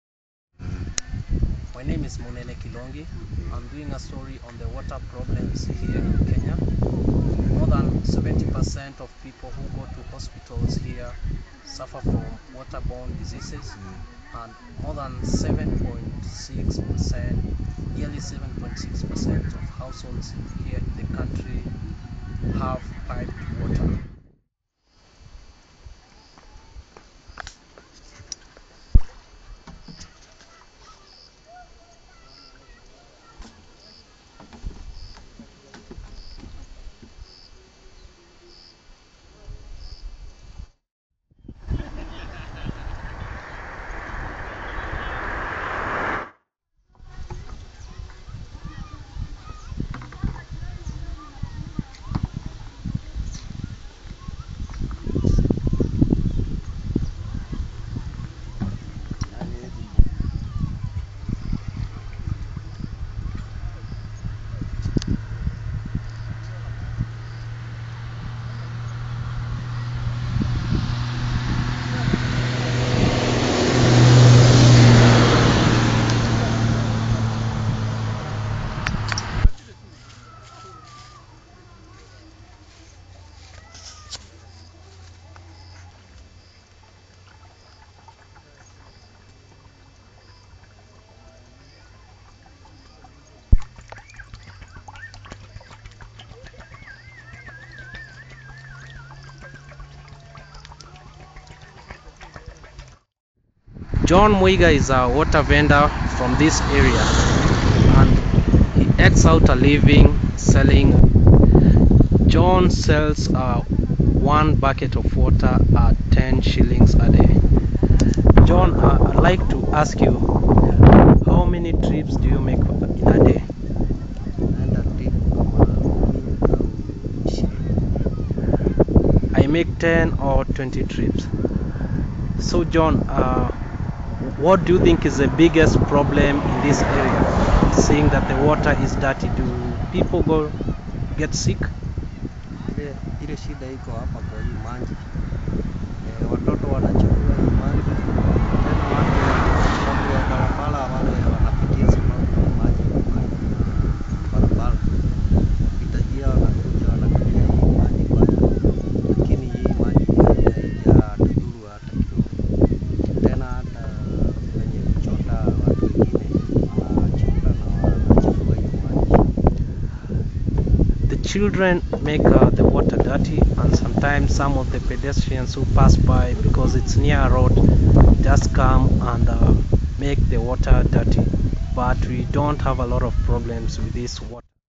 Less than 10% of Kenyans drink clean water
Munene Kilongi, Nairobi, Kenya (22 December 2007) Water is a scarce commodity in Africa and Kenya is among the countries that it is a big problem. Most people's source of water come from boreholes, rivers, streams and wells with some statistics pointing out nearly 8 percent of Kenyans have access to piped water.
Global warming, urban runoff, industrial waste and destruction of water catchment areas, and illegal loggings are some of the causes of decline of water quality and volume.